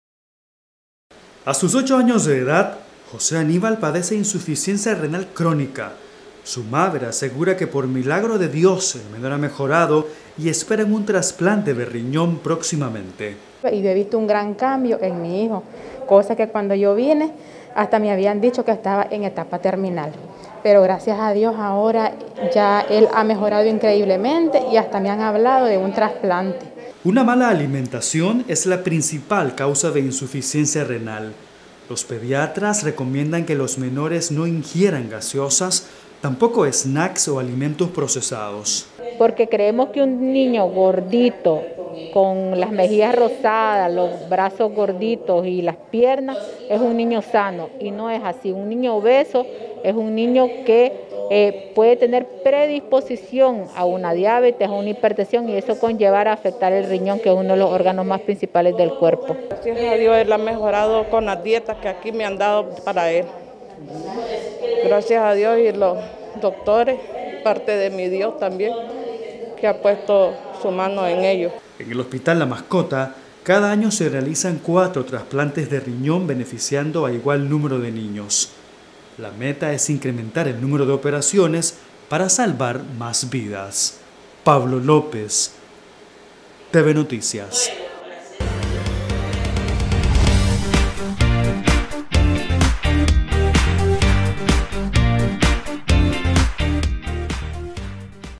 Mala nutrición: primera causa de enfermedad renal en niños
Una mala nutrición, es la principal causa de enfermedad renal en niños, así lo indicaron autoridades del Ministerio de Salud (Minsa), las declaraciones se dan en el marco de la conmemoración del Día Mundial del RiñónPara evitar las enfermedades renales, es importante que los niños no ingieran gaseosas u otras bebidas azucaradas, los snacks y alimentos procesados tampoco son recomendados por los pediatras del hospital La Mascota, en este centro de referencia nacional se cuentan con 7 máquinas para realización de hemodiálisis a los menores.